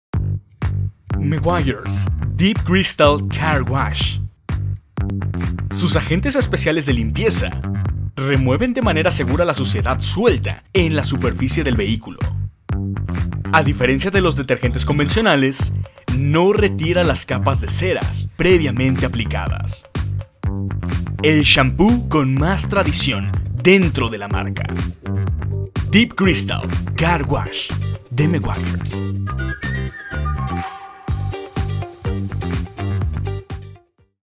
Meguiar's Shampoo Deep Crystal G10464
Deep Crystal Shampoo con sus ingredientes especiales de espuma remueven de manera segura la suciedad y la mugre de la superficie de tu auto. A diferencia de los detergentes no retira las capas de cera gracias a su pH neutro. El Shampoo con más tradición de Meguiars.